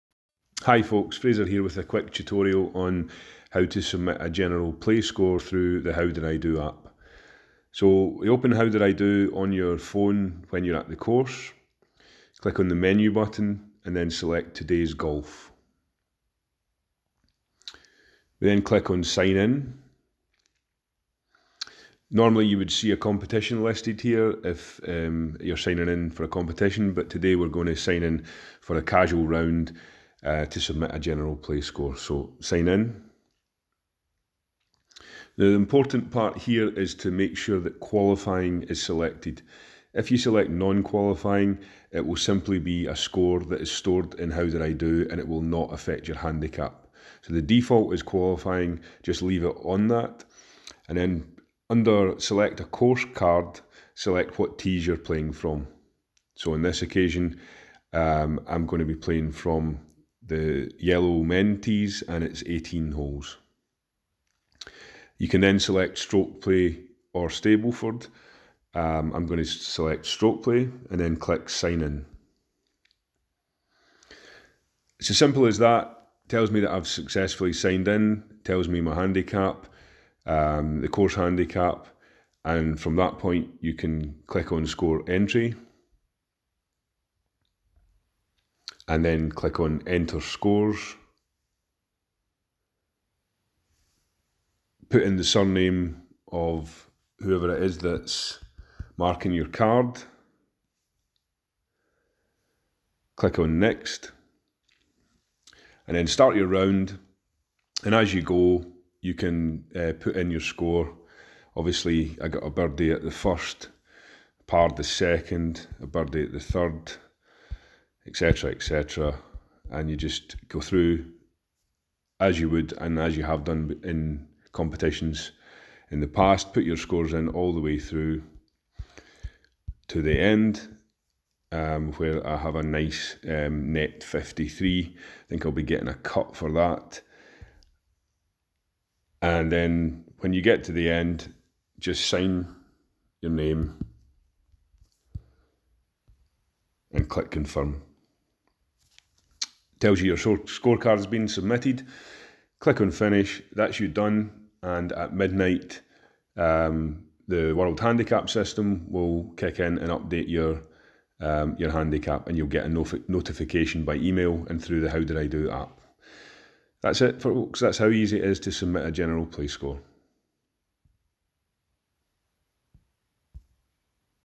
how to submit a general play score on HDID
Short tutorial on how to submit a general play score using the HowDidiDo app